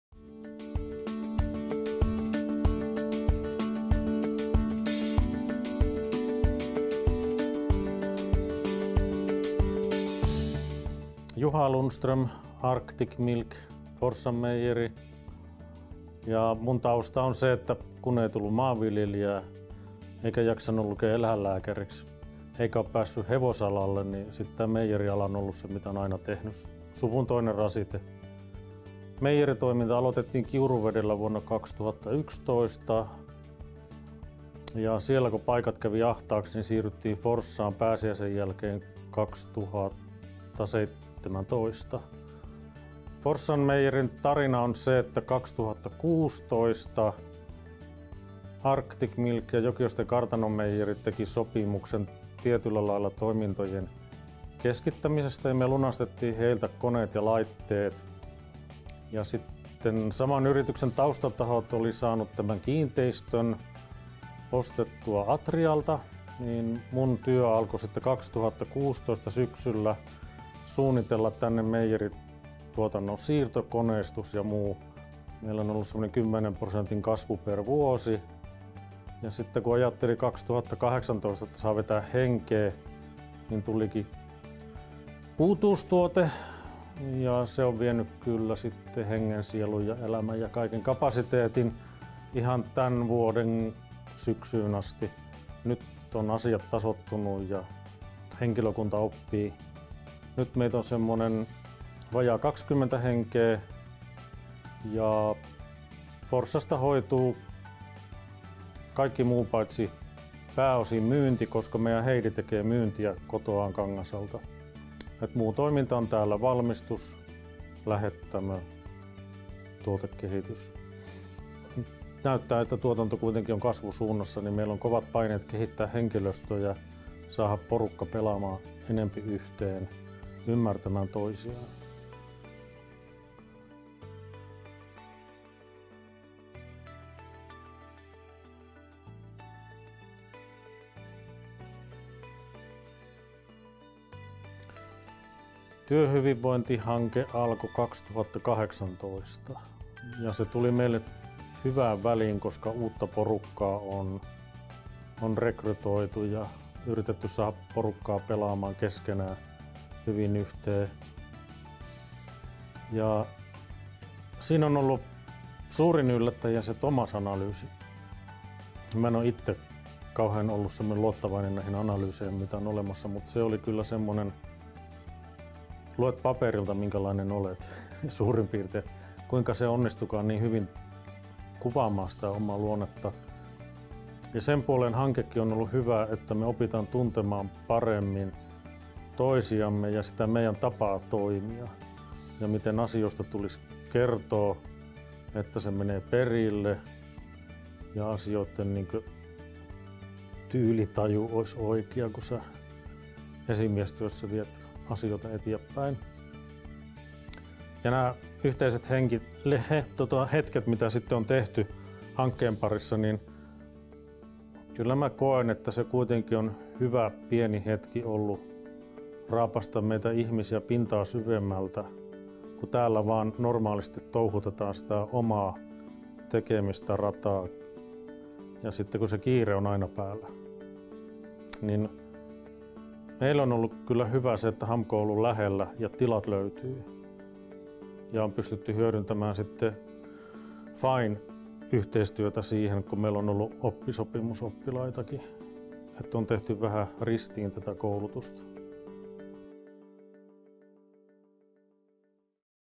Arctic Milk - Innostu työssä! -hanke
Arctic Milk-meijerin toimitusjohtaja Juha Lundström kertoo Forssan meijerin toiminnasta ja miten Innostu työssä! -hanke on auttanut heitä.